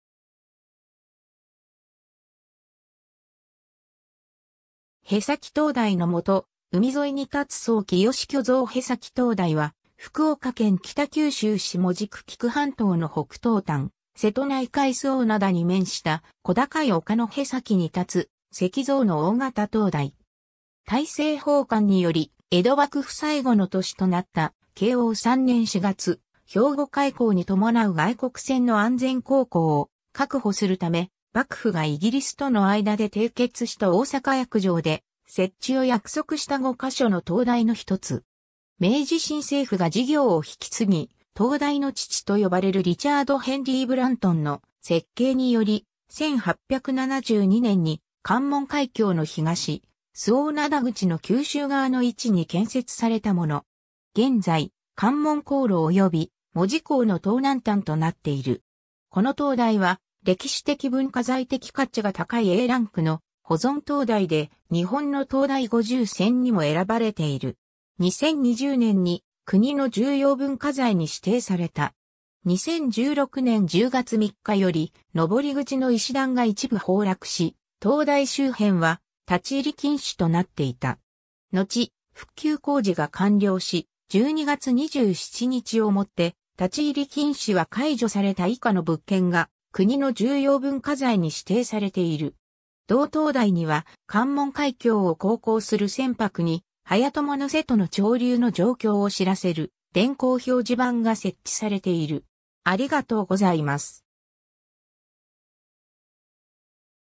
部埼灯台
部埼灯台の下、海沿いに建つ僧清虚像（2017年9月） 部埼灯台（へさきとうだい）は、福岡県北九州市門司区企救半島の北東端、瀬戸内海周防灘に面した小高い丘の部埼に立つ、石造の大型灯台。
 大政奉還により江戸幕府最後の年となった慶応3年4月（1867年）、兵庫開港に伴う外国船の安全航行を確保するため、幕府がイギリスとの間で締結した大坂約定（大坂条約）で設置を約束した五カ所の灯台の一つ。
明治新政府が事業を引き継ぎ、「灯台の父」と呼ばれるリチャード・ヘンリー・ブラントンの設計により、1872年に関門海峡の東・周防灘口の九州側の位置に建設されたもの。
現在、関門航路及び門司港の東南端となっている。
 この灯台は、歴史的文化財的価値が高いAランクの保存灯台で、日本の灯台50選にも選ばれている。
2020年に国の重要文化財に指定された。
 2016年10月3日より、登り口の石段が一部崩落し灯台周辺は立入禁止となっていた。
のち、復旧工事が完了し、12月27日をもって立入禁止は解除された 以下の物件が国の重要文化財に指定されている。
 同灯台には関門海峡を航行する船舶に早鞆瀬戸の潮流の状況を知らせる電光表示板（部埼潮流信号所）が設置されている。